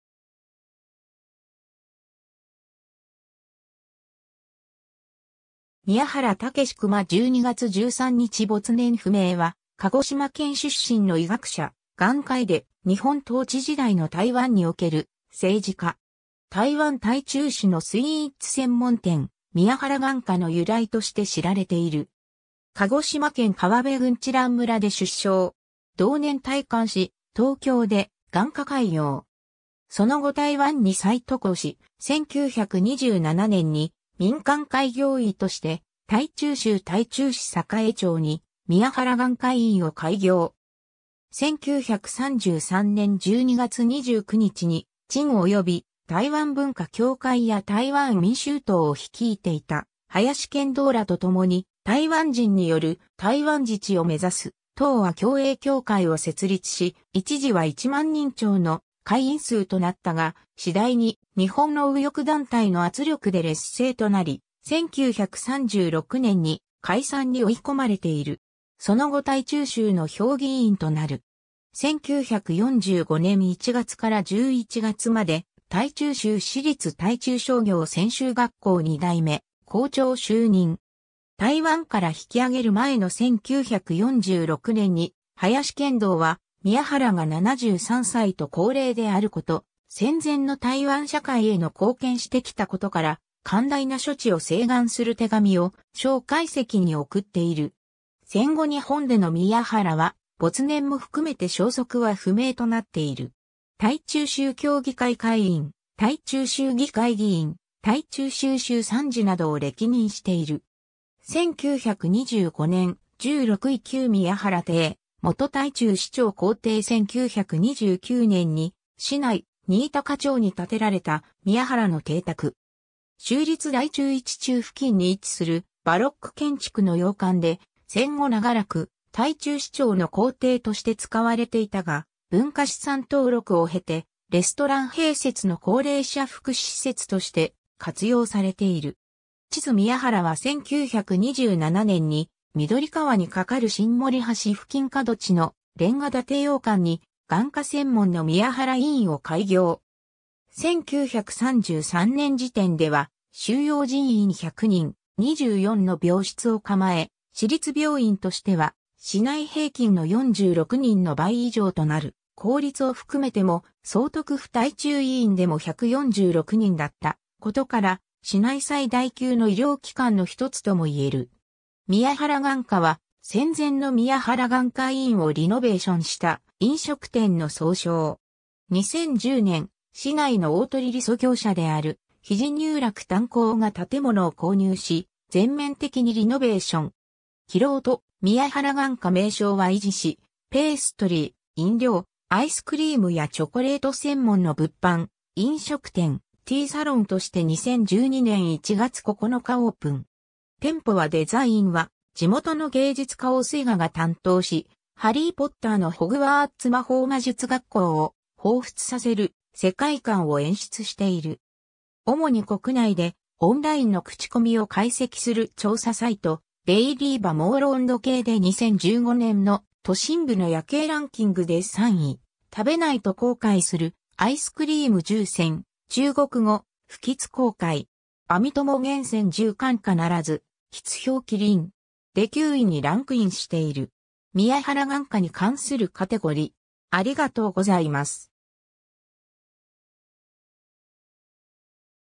宮原武熊
ルートヴィヒ・マクシミリアン大学ミュンヘン
宮原 武熊（みやはら たけくま、1874年（明治7年）12月13日 - 没年不明）は鹿児島県出身の医学者、眼科医で、日本統治時代の台湾における政治家。
台湾台中市のスイーツ専門店「宮原眼科（日出宮原眼科）」の由来として知られている。
 鹿児島県川辺郡知覧村（現・南九州市）で出生。
 同年退官し、東京で眼科開業。
その後台湾に再渡航し、1927年（昭和2）に民間開業医として台中州台中市栄町に宮原眼科医院を開業。
 1933年（昭和8）12月29日に陳炘および台湾文化協会や台湾民衆党を率いていた林献堂らとともに台湾人による台湾自治を目指す『東亜共栄協会』を設立し、一時は1万人超の会員数となったが、次第に日本の右翼団体の圧力で劣勢となり、1936年に解散に追い込まれている。
 その後台中州の評議員となる。
 1945年1月から11月まで台中州私立台中商業専修学校（現・私立新民高級中学）2代目校長就任。
 台湾から引き揚げる前の1946年に林献堂は、宮原が73歳と高齢であること、戦前の台湾社会への貢献してきたことから寛大な処置を請願する手紙を蔣介石に送っている。
戦後日本での宮原は没年も含めて消息は不明となっている。
 台中州協議会会員、台中州議会議員、台中州州参事などを歴任している。
 1925年：従六位 旧宮原邸、元台中市長公邸 1929年（昭和4）に市内新高町（現・北区雙十路一段）に建てられた宮原の邸宅。
州立台中一中（現・台中市立台中第一高級中等学校）付近に位置するバロック建築の洋館で、戦後長らく台中市長の公邸として使われていたが、文化資産登録を経てレストラン併設の高齢者福祉施設として活用されている。
 地図 宮原は1927年に緑川に架かる新盛橋付近（現在の中山路と緑川東街交差点にある中山緑橋）角地の煉瓦建て洋館に眼科専門の宮原醫院を開業。
1933年（昭和8）時点では収容人員100人、24の病室を構え、私立病院としては市内平均の46人の倍以上となる、 公立を含めても総督府台中医院でも146人だったことから、市内最大級の医療機関の一つともいえる。
 宮原眼科は戦前の宮原眼科医院をリノベーションした飲食店の総称。
 2010年、市内の鳳梨酥（パイナップルケーキ）業者である『日出乳酪蛋糕（日出、リーチュー、Dawncake）』が建物を購入し、全面的にリノベーション。
 騎楼と「宮原眼科」名称は維持し、ペイストリー（糕點）、飲料、アイスクリーム（冰淇淋）やチョコレート（巧克力）専門の物販、飲食店（2階の台湾料理レストラン『醉月樓沙龍』）、ティーサロンとして2012年1月9日オープン。
店舗はデザインは地元の芸術家王水河（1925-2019）が担当し、ハリー・ポッターのホグワーツ魔法魔術学校を彷彿させる世界観を演出している。
 主に国内でオンラインの口コミを解析する調査サイト『DailyView 網路温度計』で2015年の「都心部の夜景ランキング（中国語: 在城市的中心呼喊無敵夜景！2015十大熱門夜拍景點）」で3位、「食べないと後悔するアイスクリーム10選（中国語: 不吃後悔！網友嚴選十款必吃冰淇淋！」で9位にランクインしている。 宮原眼科に関するカテゴリ: